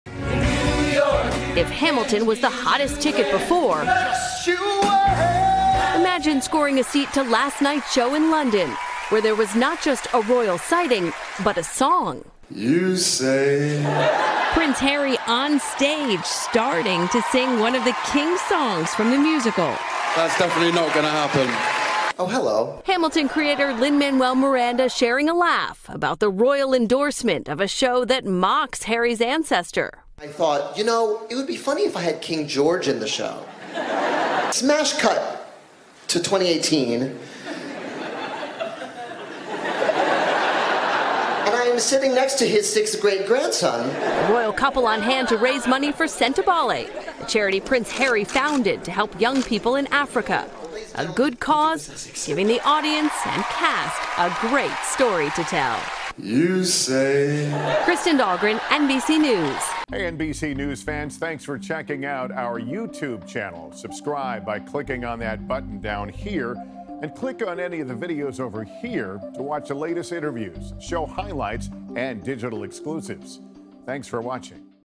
Prince Harry Sings At Charity Performance Of ‘Hamilton’ In London | NBC Nightly News
The Prince surprised the cast and audience, singing one of the musical’s songs after a performance Wednesday night.